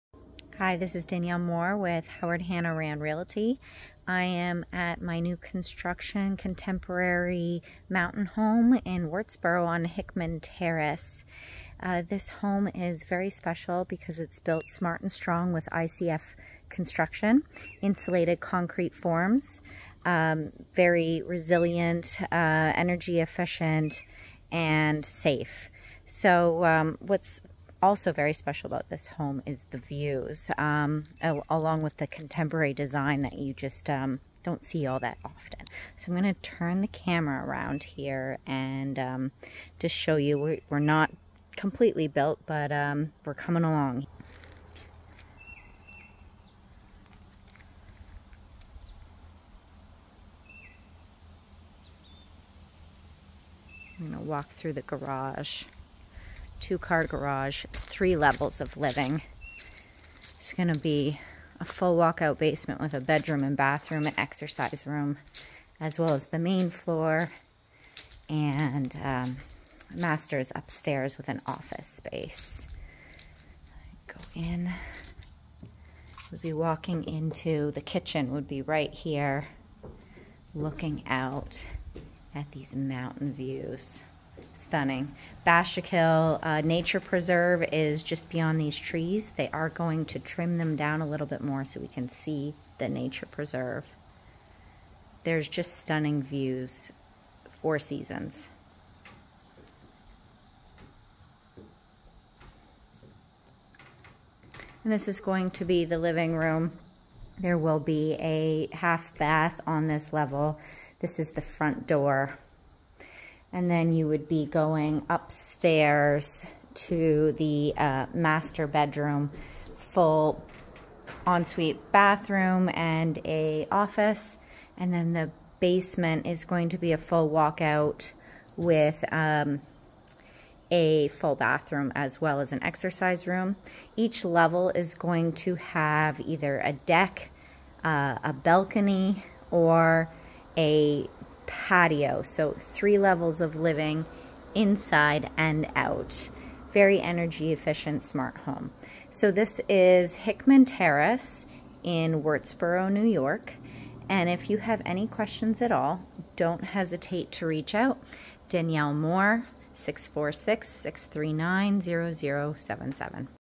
Modern Mountain Home 🏡
Built Strong and Smart with ICF Construction. 6 Acres. Stunning Views and Privacy. This energy efficient contemporary mountain home has 3 levels of living inside and out with a sleek and well thought out design.

Still time to customize. Estimated completion Oct 2022.

Hickman Terrace Rd Wurtsboro, NY
HH Rand Realty